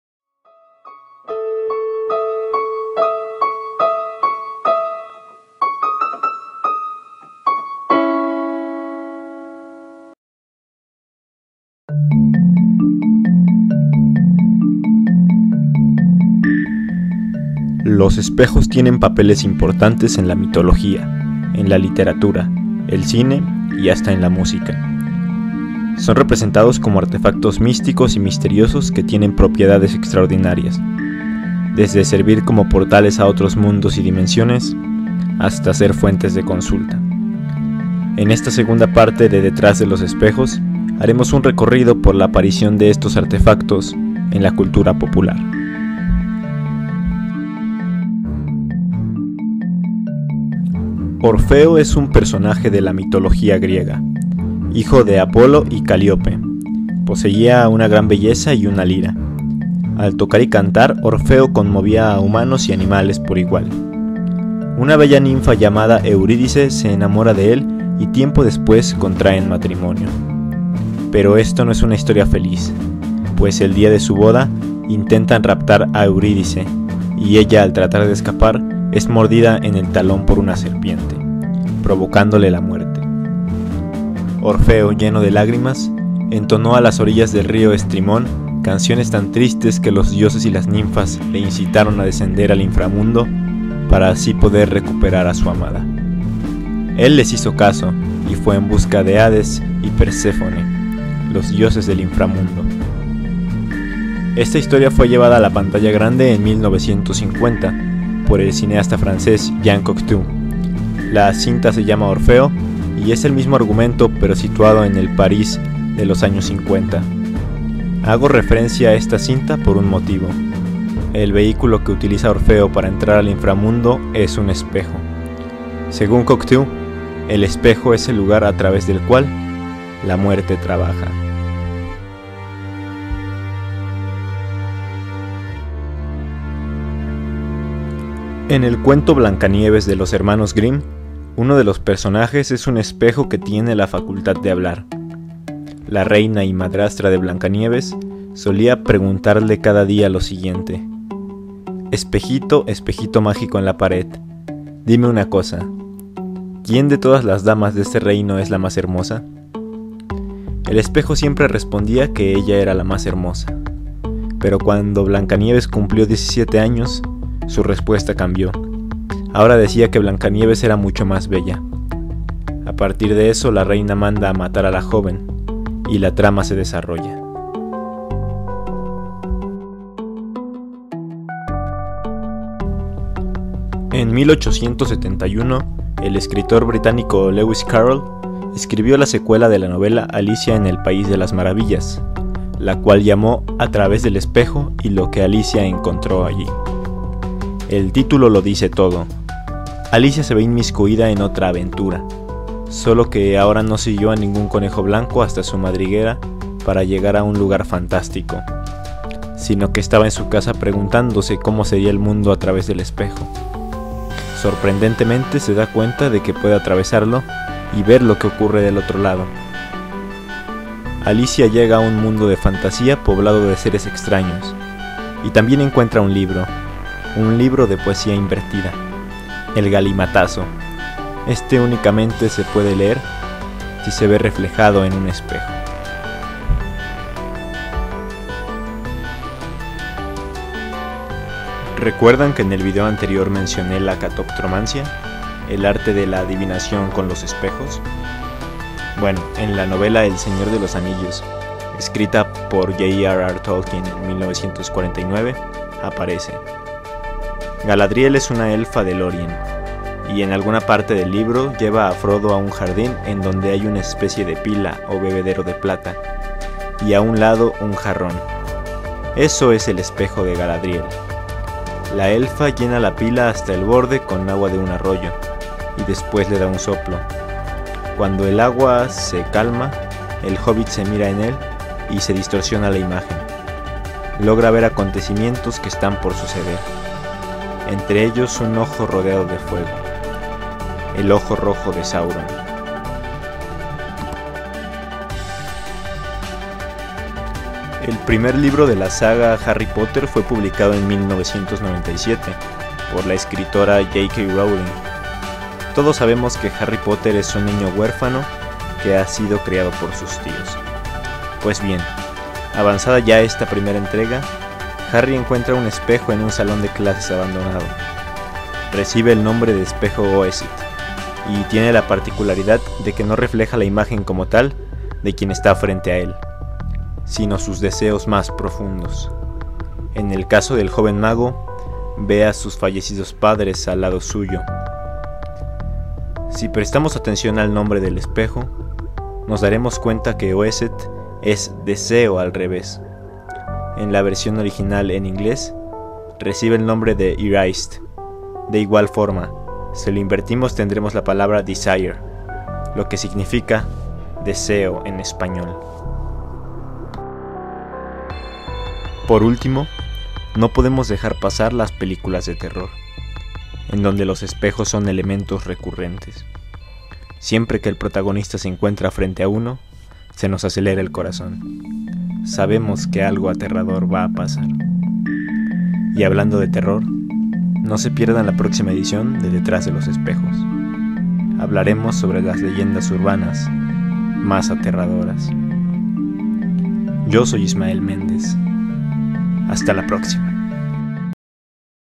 Detrás de los espejos (2/3): Cine y literatura
Los espejos tienen papeles importantes en la mitología, en la literatura y en el cine y hasta en la música. Son representados como artefactos místicos y misteriosos que tienen propiedades extraordinarias.